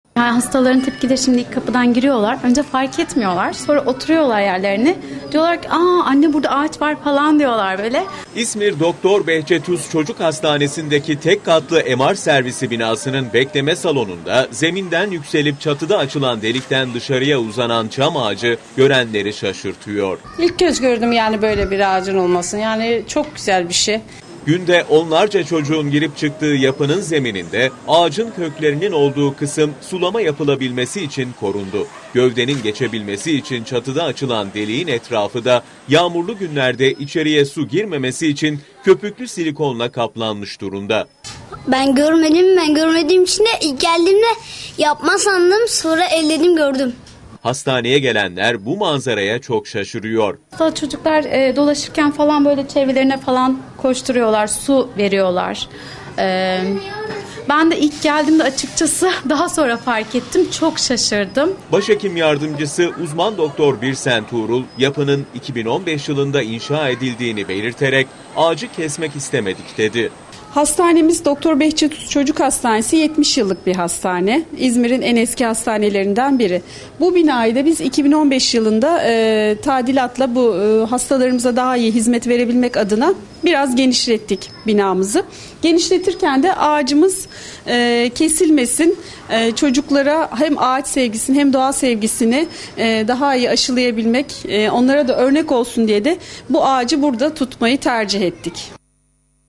İZMİR'DE HASTANE İÇİNDEKİ AĞAÇ GÖRENLERİ ŞAŞIRTIYOR! | YOK DAHA NELER
İzmir'de Hastane içerisindeki ağaç görenleri şaşkınlığa uğratıyor!